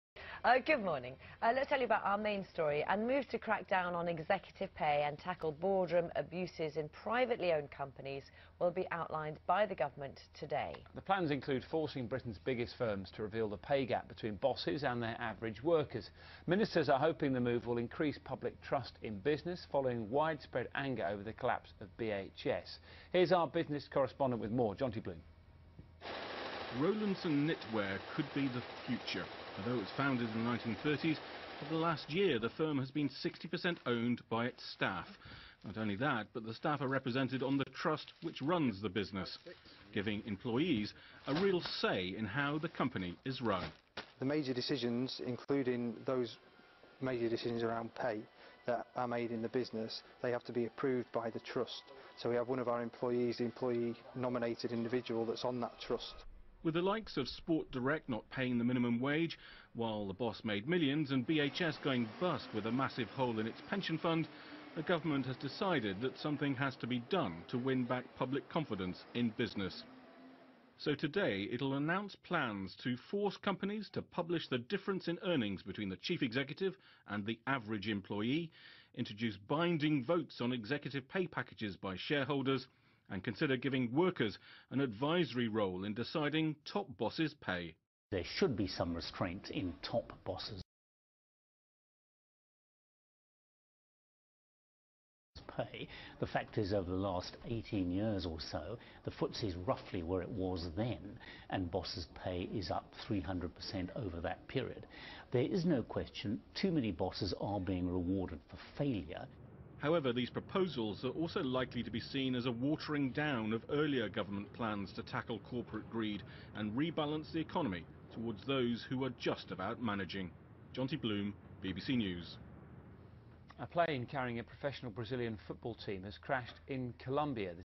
Rowlinson Knitwear - BBC interview with Neil Ward 2016
Director of Rowlinson Knitwear, Neil Ward, interviewed by the BBC about Theresa May's plans to address corporate greed and the pay gap between workers and executives.